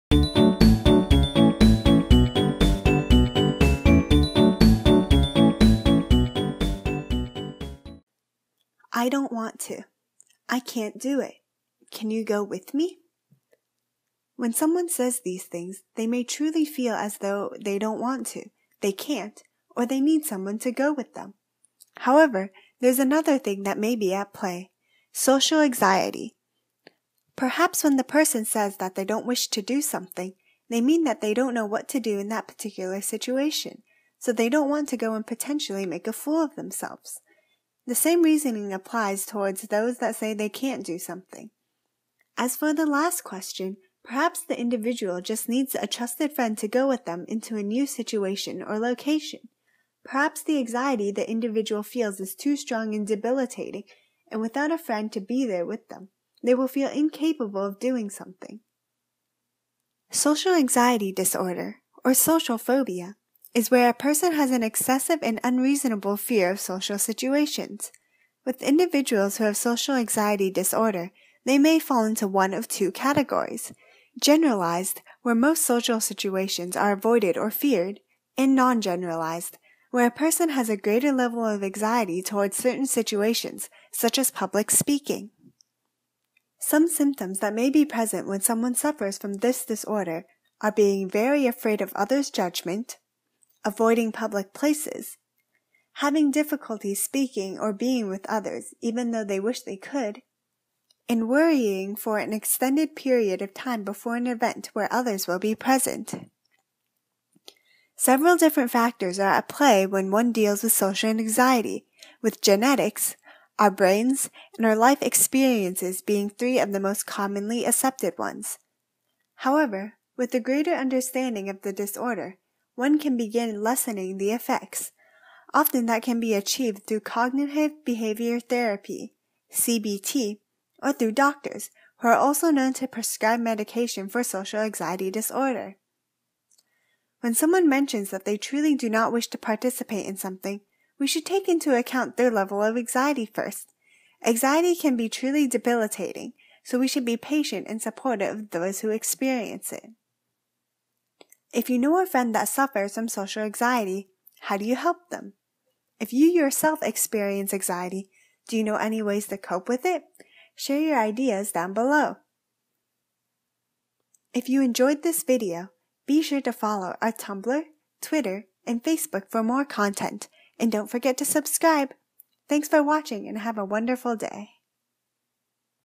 Social Anxiety - More than Shyness
When it comes to social anxiety, it's more than just being shy. Social anxiety is the fear of social situations that involve interaction with other people. You could say social anxiety is the fear and anxiety of being negatively judged and evaluated by other people. It is a pervasive disorder and causes anxiety and fear in most all areas of a person's life. Whereas being shy is more of a personality trait. You can be shy but not have social anxiety or feel anxiety around people.

Let us know if you agree!